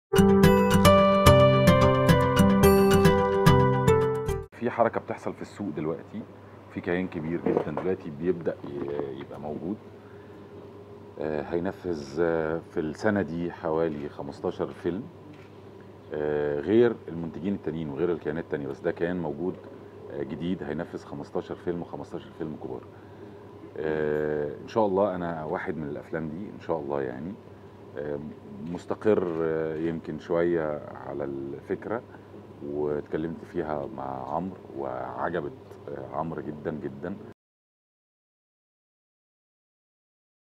طارق لطفي: كيان كبير في السوق هينفذ إنتاج ١٥ فيلم السنة دي
تصوير: روفيدة جمال